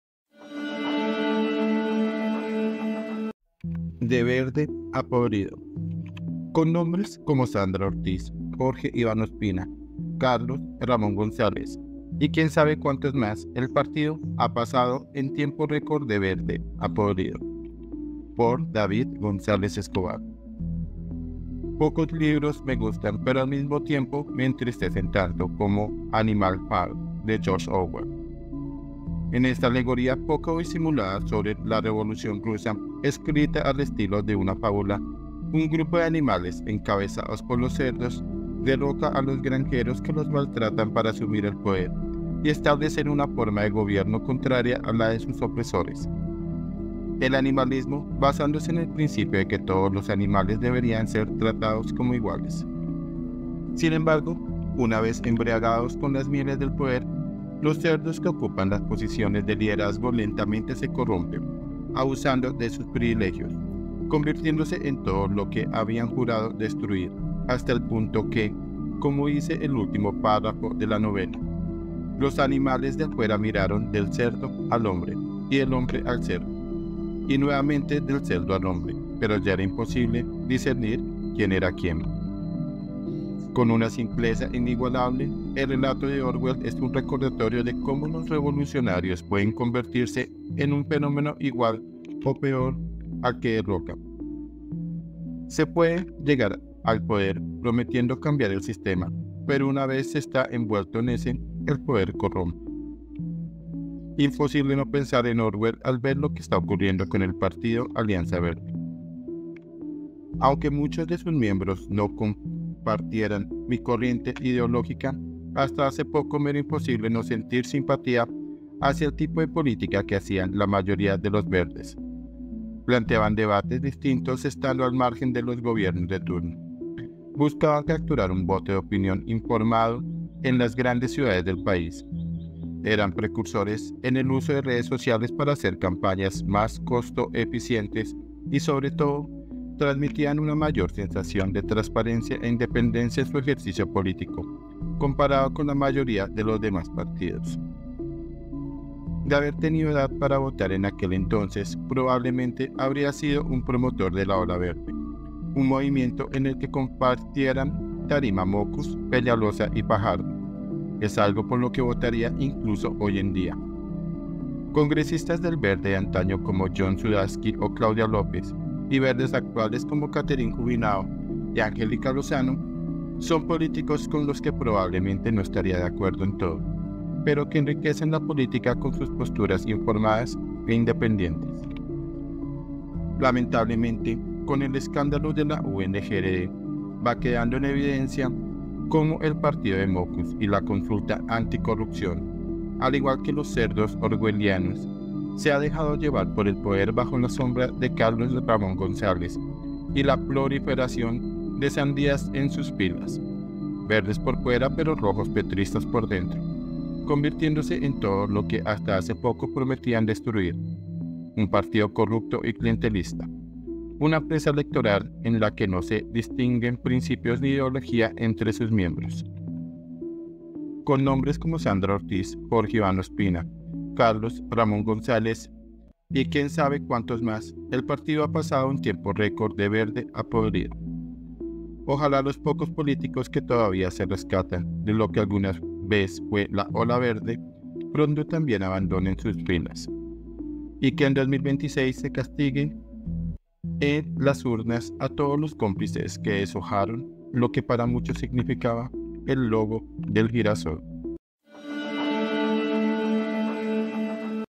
DE VERDE A PODRIDO 🟢 Por David González Escobar
Con nombres como Sandra Ortiz, Jorge Iván Ospina, Carlos Ramón González y quién sabe cuántos más, el partido ha pasado en tiempo récord de Verde a podrido.
columna DAVID GONZÁLEZ ESCOBAR